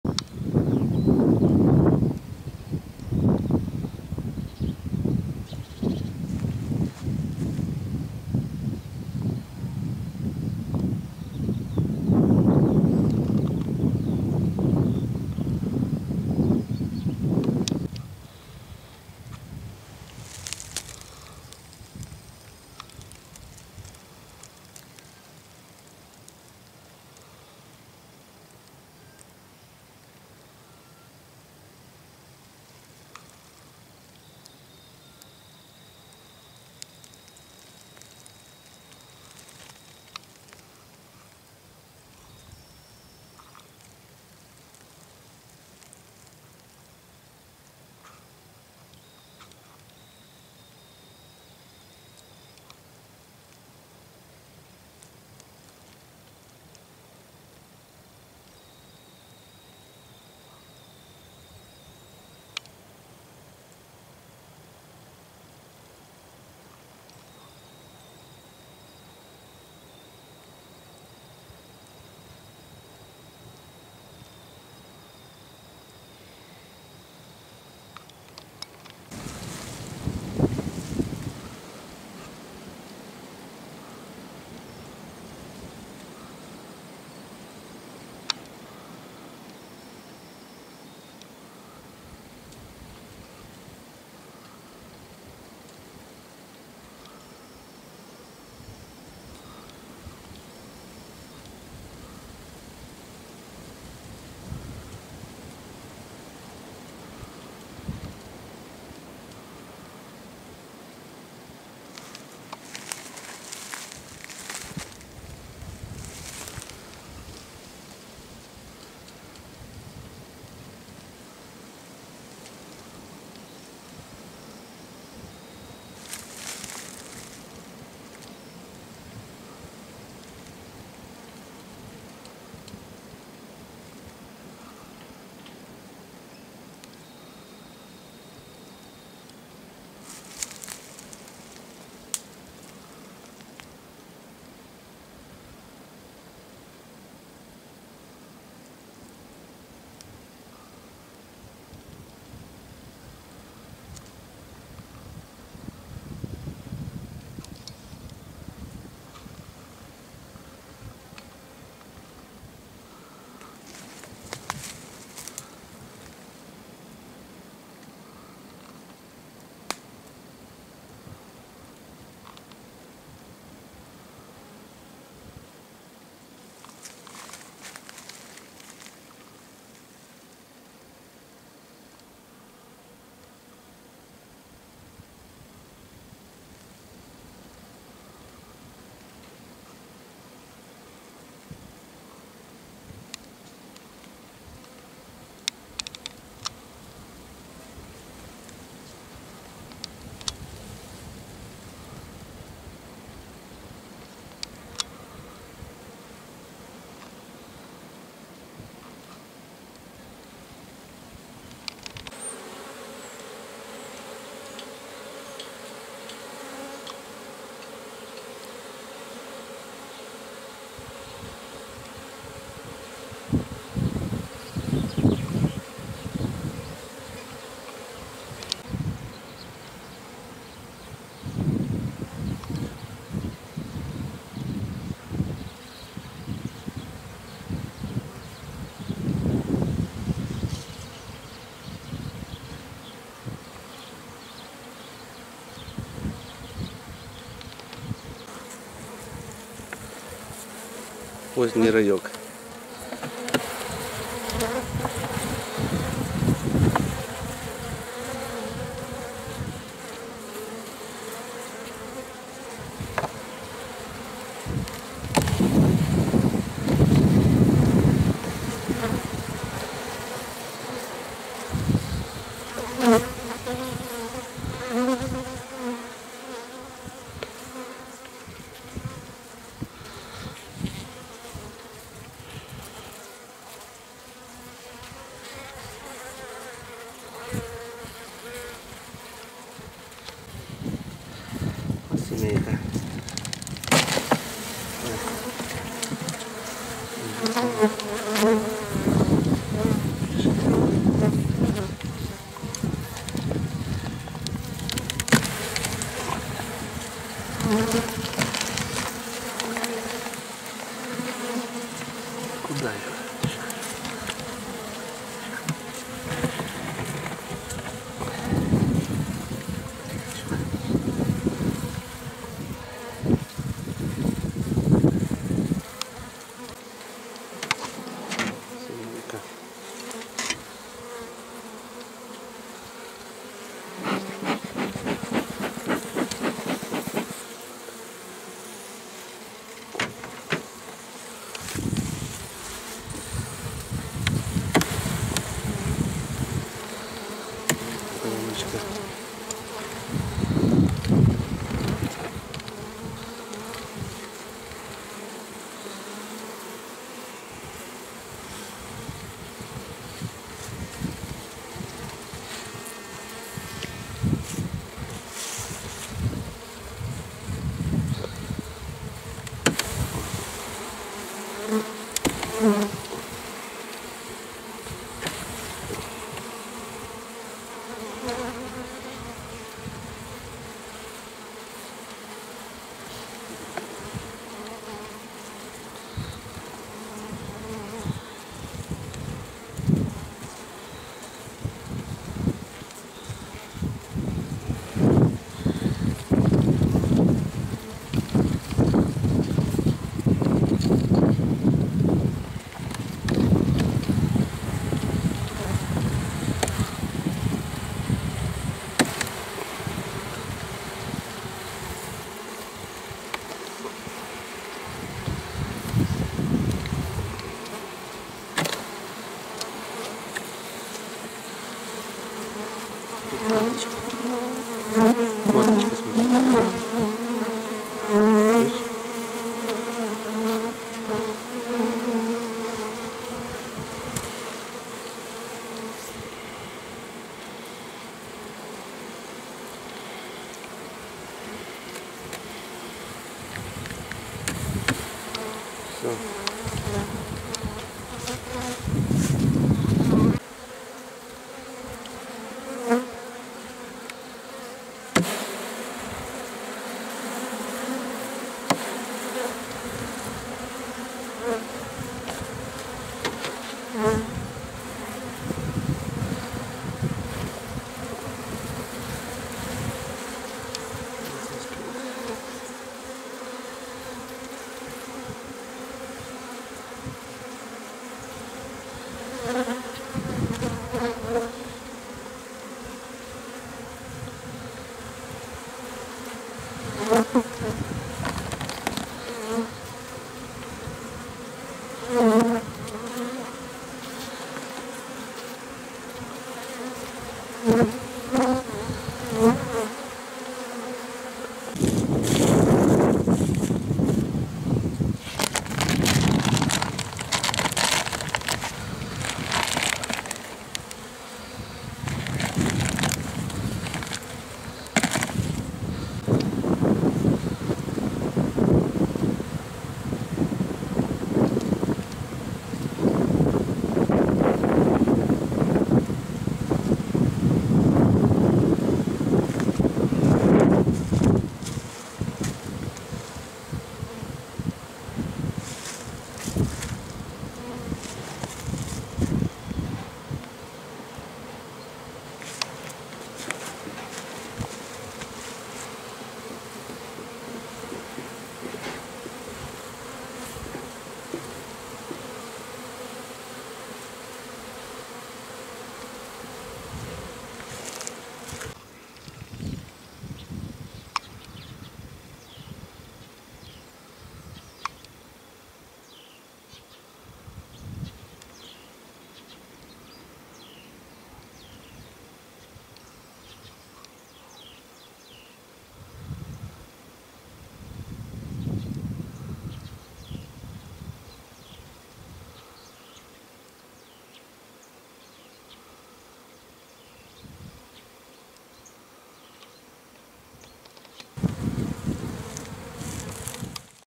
18 03 19 облёт
Второй в этом году благоприятный день для облёта. Первая пыльца. Беглый осмотр слабышей.